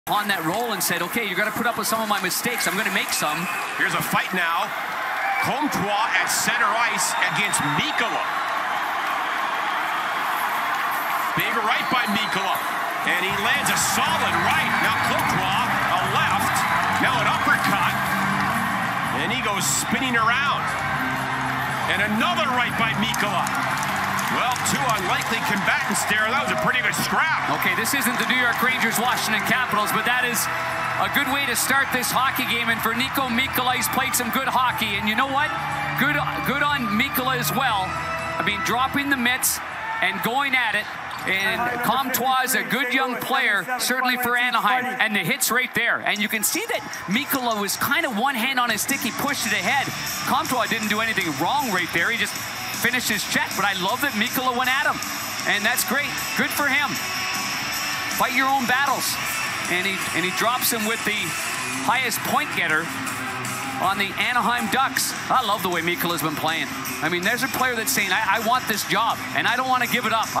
Maxime Comtois vs Niko Mikkola May 5, 2021
Maxime Comtois vs Niko Mikkola from the Anaheim Ducks at St Louis Blues game on May 5, 2021.